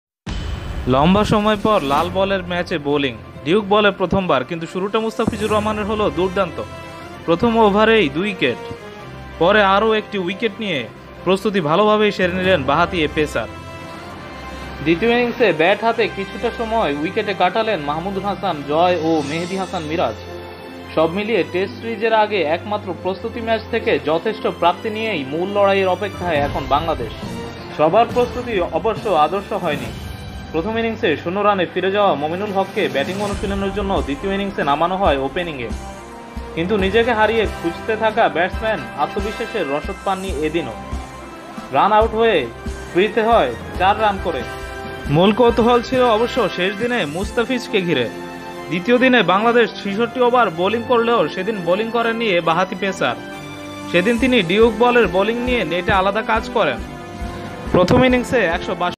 প্রেসিডেন্ট একাদশের বিপক্ষে প্রস্তুতি ভালোই নিলো বাংলাদেশ। বেশী ভালো হলো তামিম ও মুস্তাফিজের।
ওয়েস্ট ইন্ডিজে প্রেসিডেন্ট একাদশের বিপক্ষে ভালোই প্রস্তুতি নিলো বাংলাদেশ । ওপেনার তামিম ইকবাল ও মুস্তাফিজুর রহমানই সবচেয়ে ভালো প্রস্তুতি নিলেন।
তবে বরাবরের মতোই ব্যর্থ মুমিনুল হক।